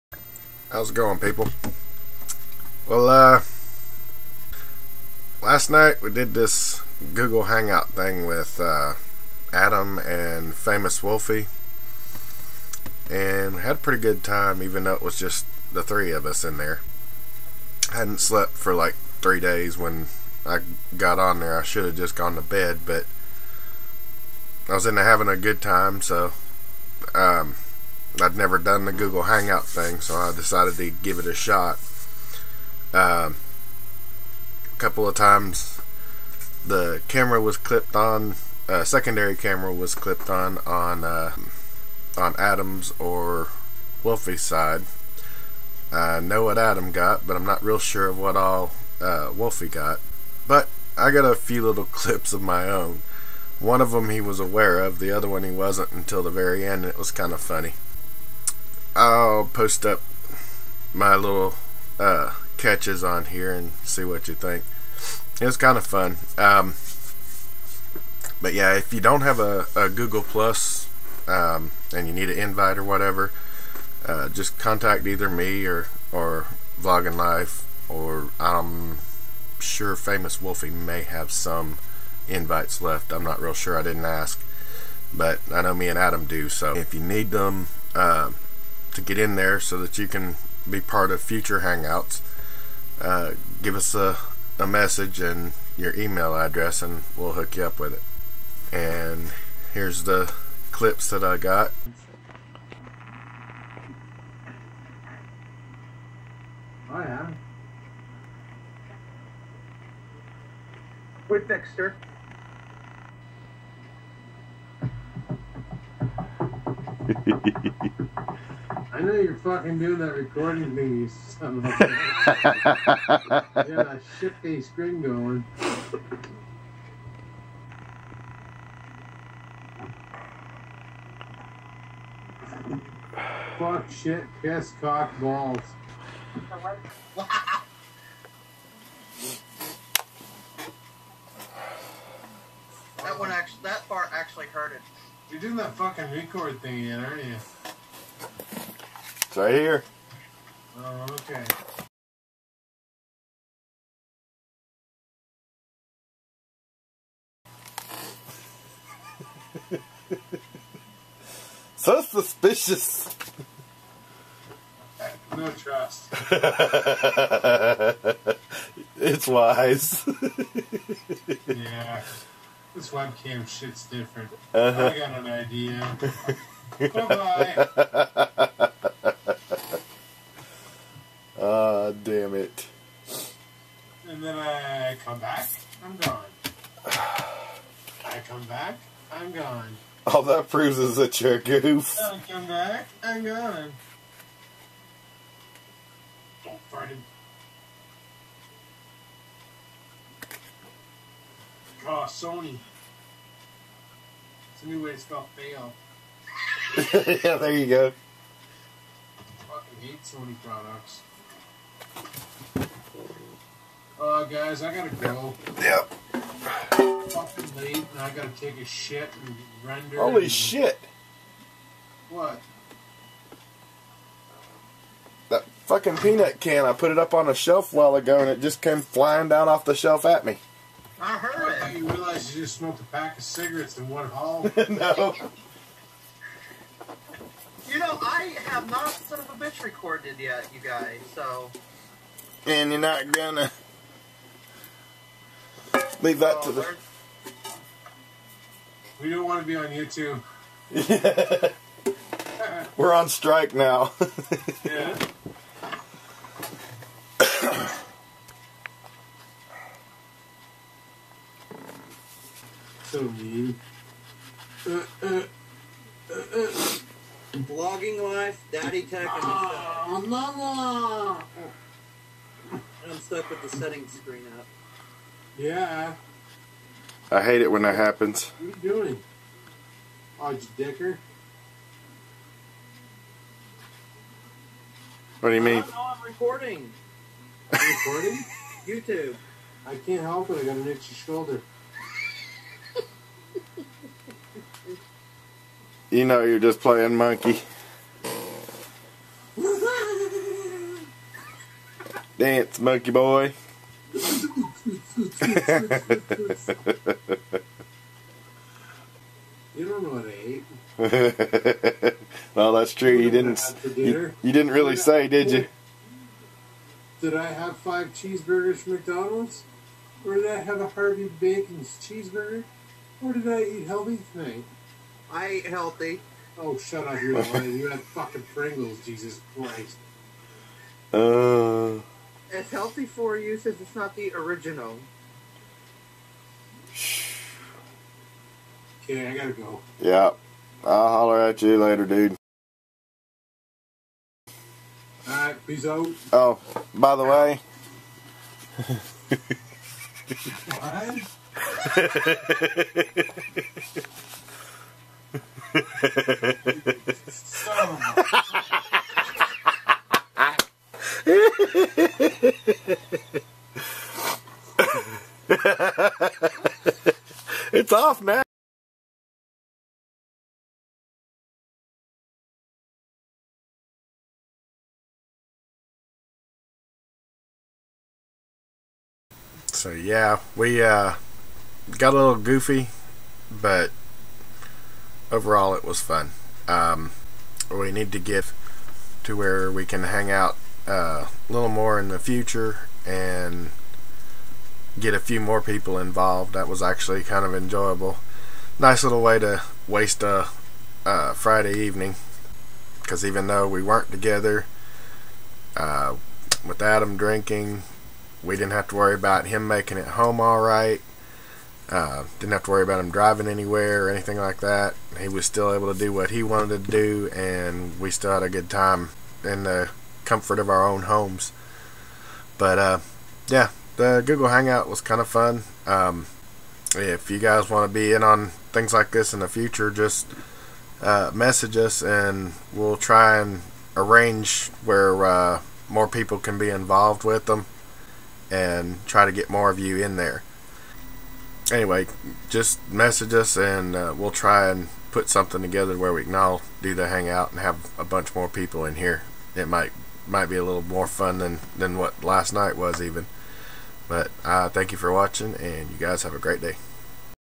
Google+ hangout
had a great time last night just hanging out on G+ being goofy for a change.